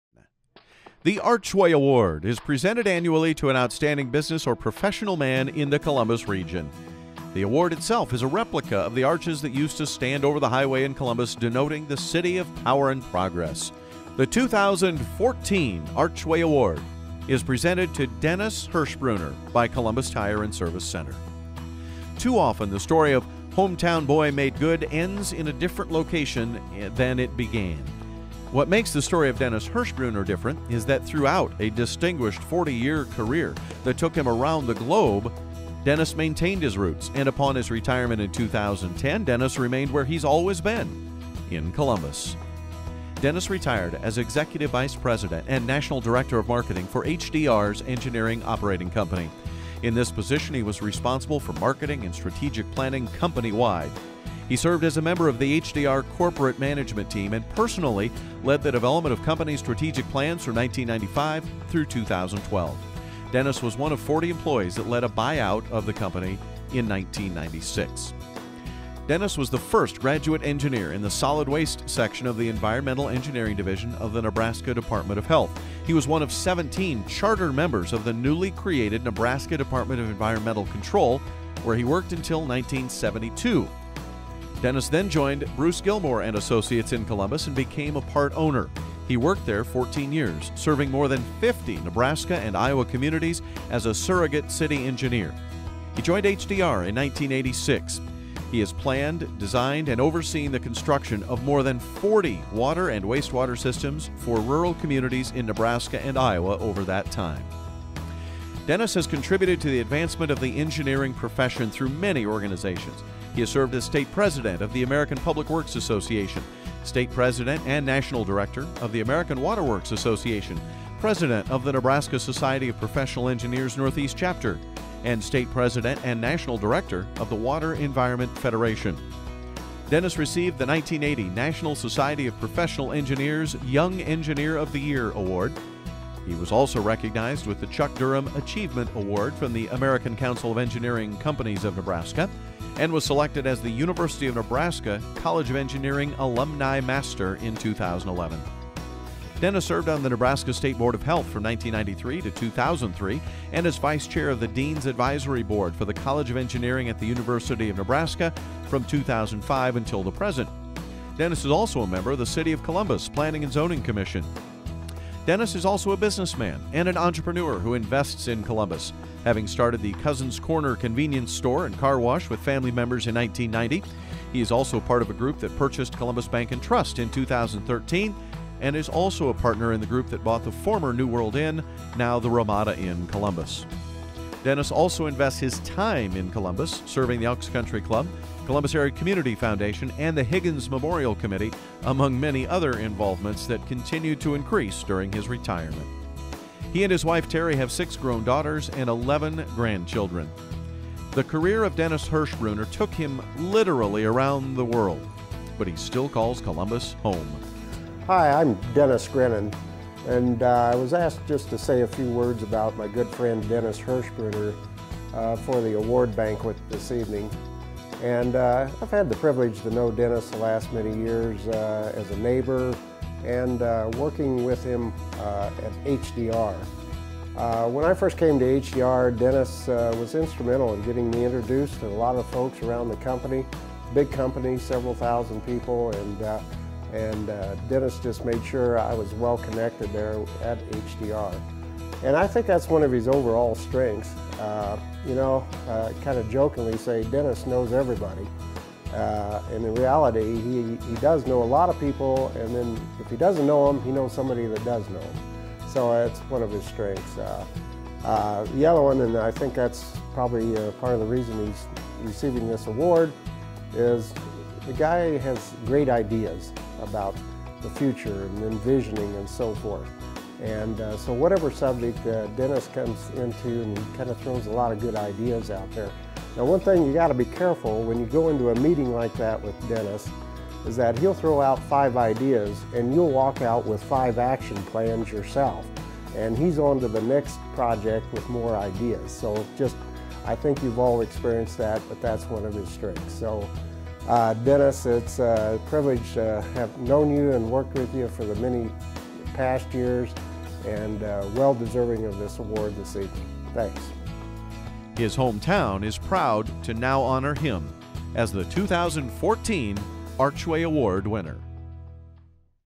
2014 Archway Award Dennis Hischbrunner
Award for Businessperson of the Year from the Columbus Area Chamber